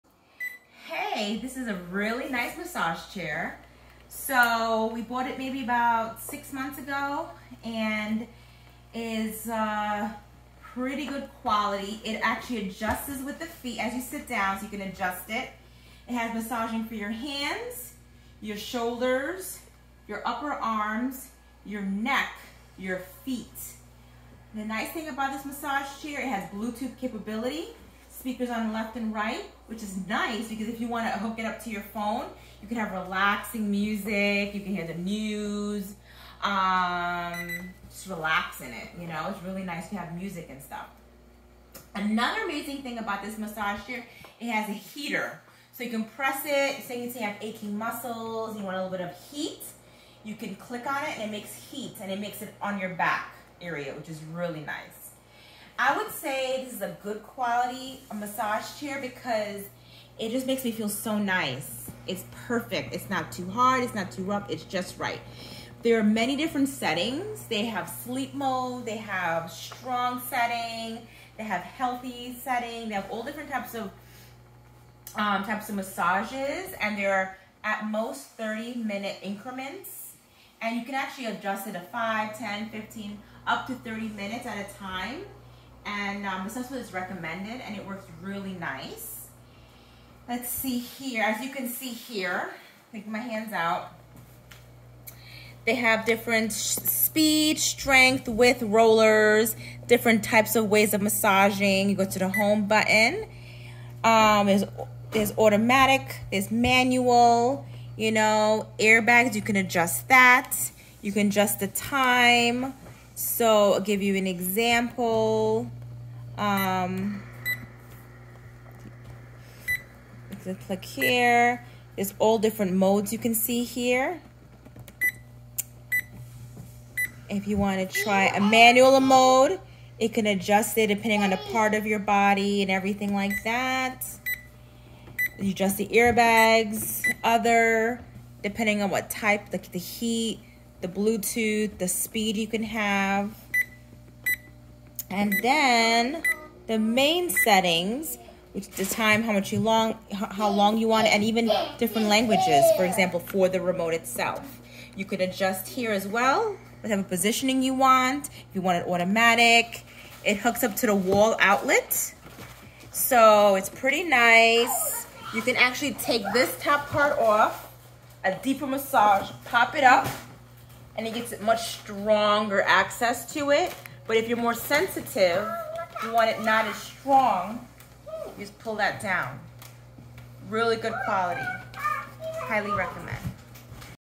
BEST MASSAGE CHAIR ON AMAZON! Budget Full Body Zero Gravity With Heat & Bluetooth | Ugears B-L1
Massage Chair,Zero Gravity SL Track Massage Chair, Full Body Shiatsu Massage Chair Recliner with Space Saving, Auto Body Detection, Thai Stretching, Bluetooth Speaker, Heat, Foot Roller Ugears B-L1